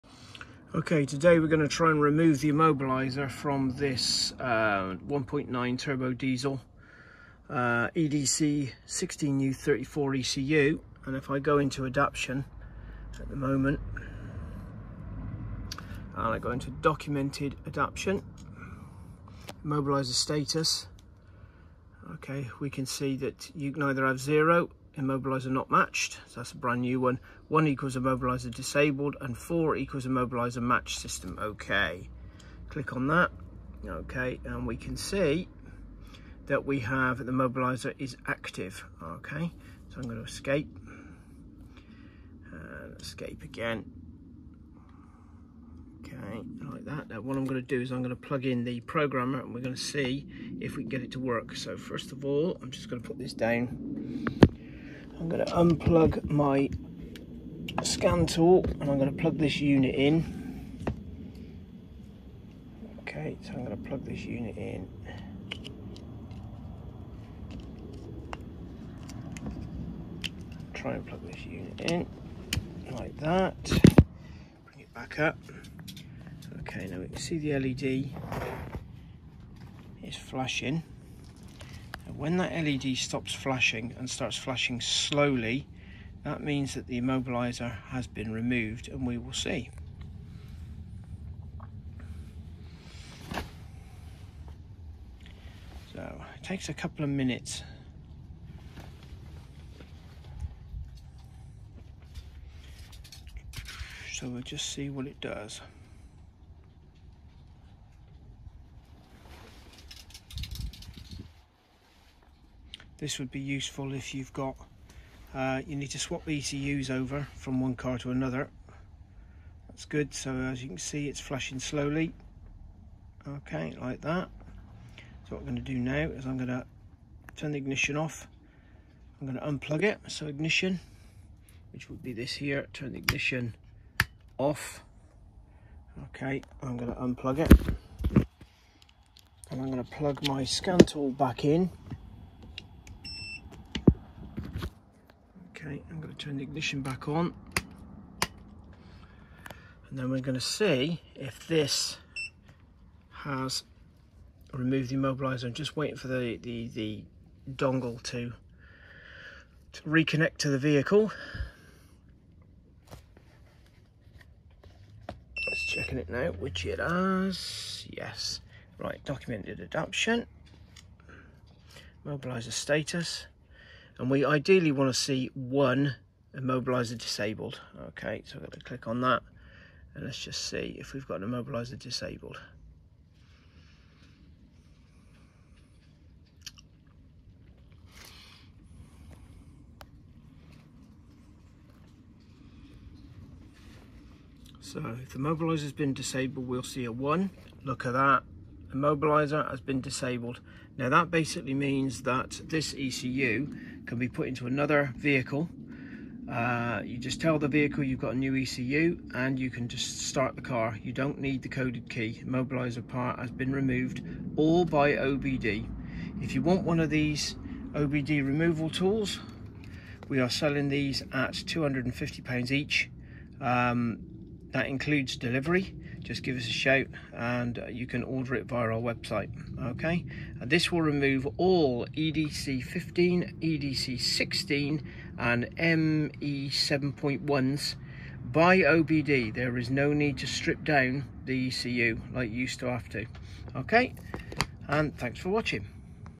vw immo off in 2 minutes from diagnostics port
Immobiliser removal by obd this is perfect if you have a car that you need to change the ecu over and cant find the pincode to program in a new key or a faulty immobiliser.
The ecu shown is a replacement for a no comms issue with an old ecu on a 2008 vw golf mk5 we have the one in the video for sale at £199 uk delivery only